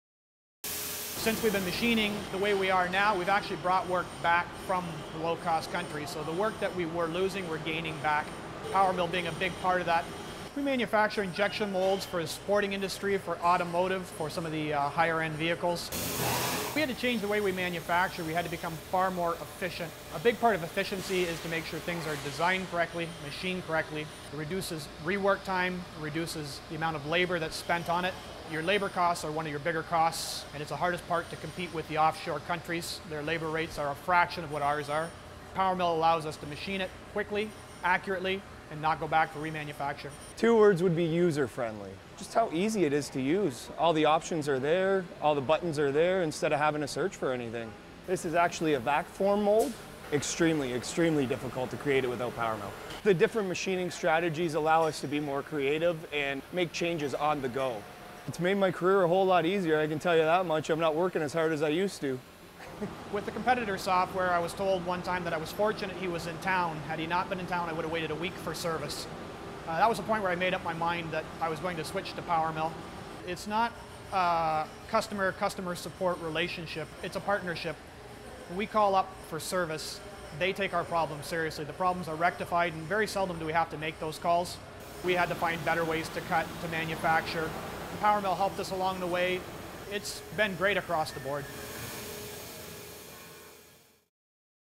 Manufacturing Injection Molds with PowerMILL at NeoVision Technology
NeoVision Technology manufacture injection moulds for the sports and automotive industries.  Find out how Delcam PowerMILL has helped them compete with lower labour costs overseas to bring back work from lower cost countries.